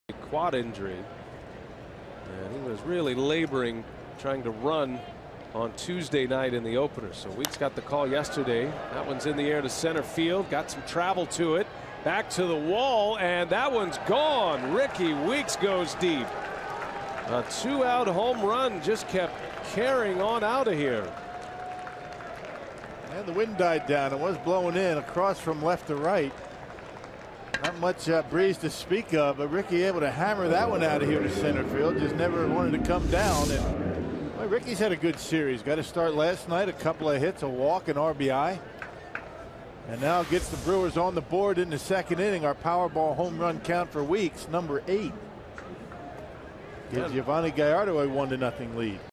MIL@CIN: Weeks blasts a solo shot to open scoring
9/25/14: Rickie Weeks belts a solo home run to center field to open the scoring and put the Brewers up 1-0 in the top of the 2nd inning